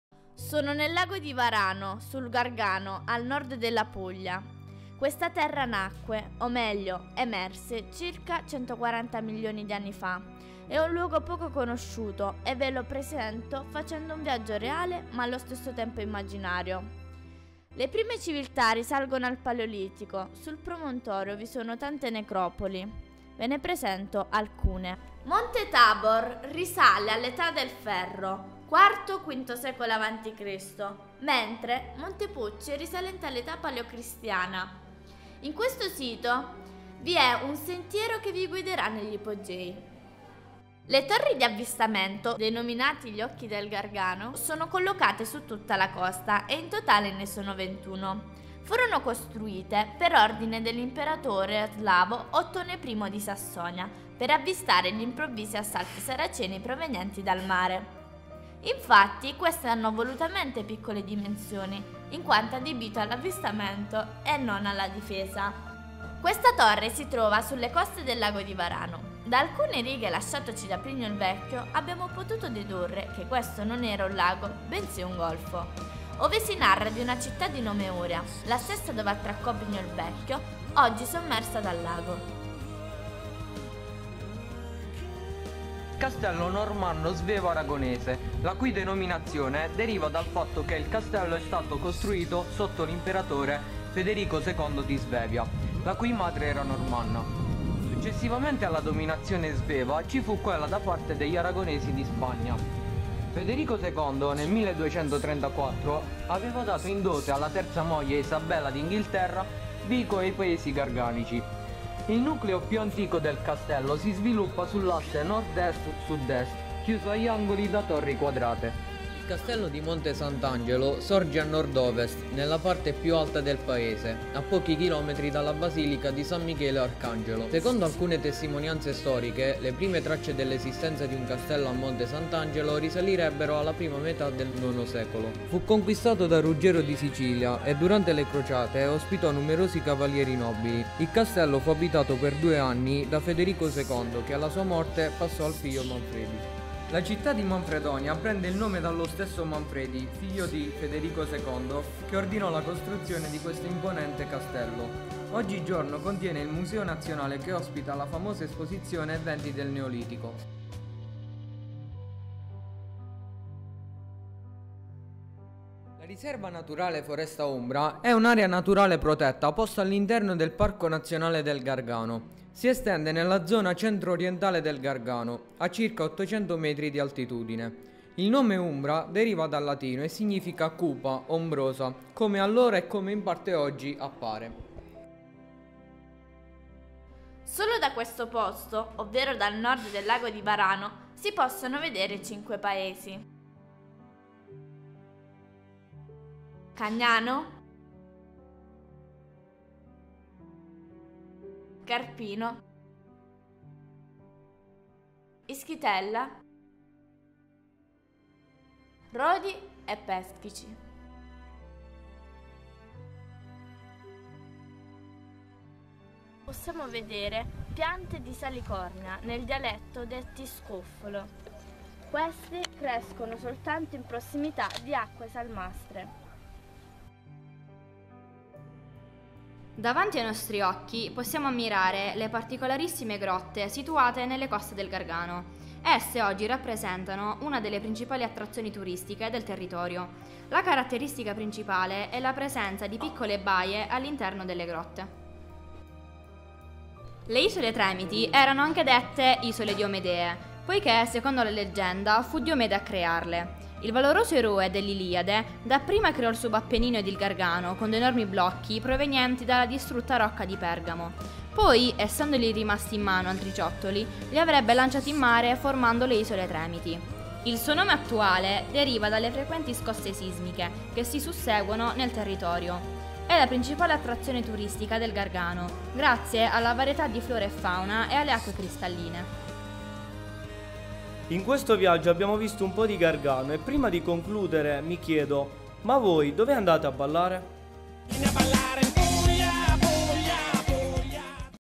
*US14111R Quel ramo del lago di Varano...
Video per il progetto Articolo 9 della Costituzione del Liceo Publio Virgilio Marone di Vico del Gargano (FG).